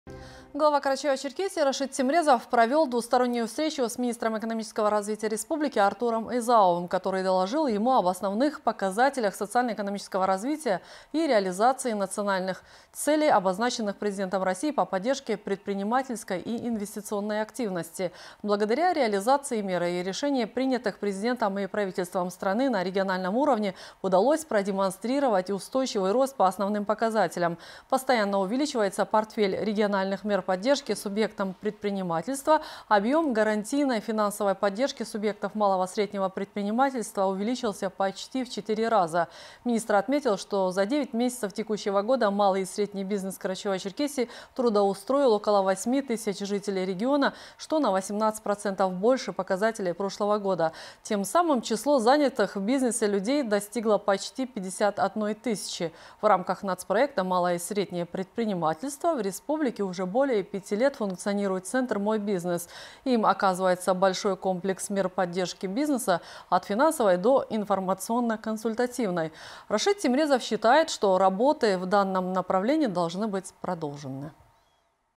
Глава КЧР обсудил с министром экономического развития республики реализацию национальных проектов
Рашид Темрезов провёл двустороннюю встречу с руководителем Минэкономразвития республики Артуром Езаовым, который сообщил о реализации обозначенных главой государства Владимиром Путиным национальных целей по поддержке предпринимательства и инвестиционной активности. Благодаря предпринятым мерам в регионе увеличивается количество рабочих мест. Так, за 9 месяцев текущего года малый и средний бизнес Карачаево-Черкесии трудоустроили около восьми тысяч человек. Хорошим подспорьем для развития предпринимательства стал открытый несколько лет назад центр «Мой бизнес» КЧР, оказывающий большой комплекс мер поддержки.
«Динамично развивая предпринимательский сектор и поддерживая наши предприятия, мы сможем расширять производства, создавать новые высокотехнологичные рабочие места, а значит, улучшать качество жизни людей», - отметил глава республики